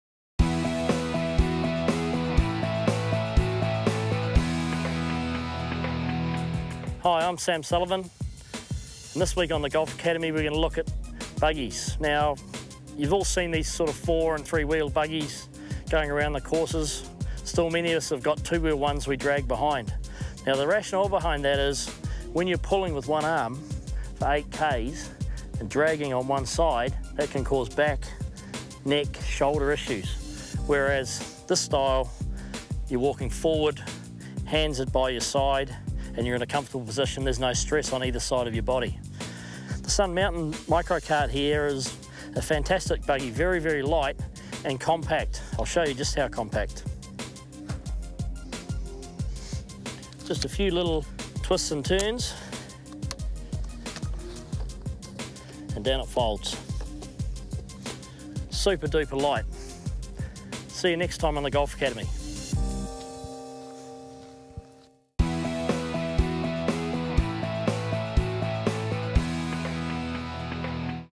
Golf Academy :: Golf Carts & Buggies
Golf technology covers not just clubs and balls but buggies and carts.  Three-wheel carts not only look great but are better for you. The Sun Mountain Micro Cart make a round feel like a breeze.